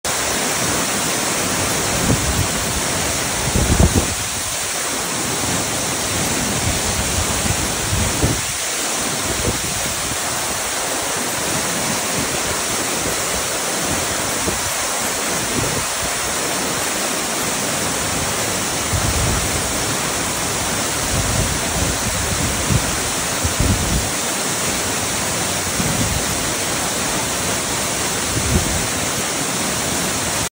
照鏡潭 Mirror Pool
照鏡潭，照鏡潭是香港最最壯觀的瀑布之一，氣勢磅礡
拍攝日因早前曾下過大雨，照鏡潭水量特別強勁